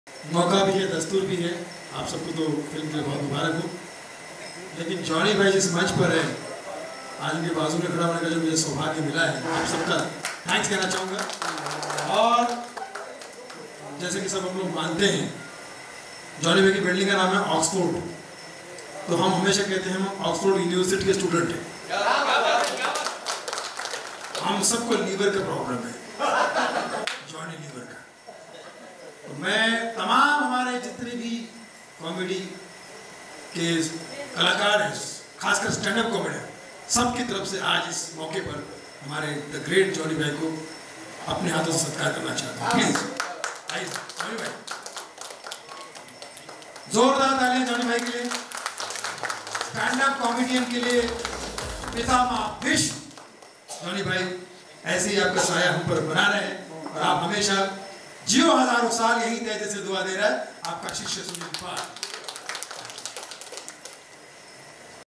Comedian Sunil Pal Speech At Ajab Tamasha Movie Trailer Launch | Johnny Lever@HindiARenterprises
Comedian Sunil Pal Speech At Ajab Tamasha Movie Trailer Launch

Director: Nayeem Ahmed
Producer: Amit Kumar Agarwal
Star Cast: Hrithik Sharma, Samar, Akbar Bin Tabar, Rubeen, Muskaan, Sajid, Tanuja, Babita and others.
Singers: Altamash Faridi, Jawed Ali
DOP: Ramu Gaduthuri
Editor: Sartaj
Production Head: Sana Shaik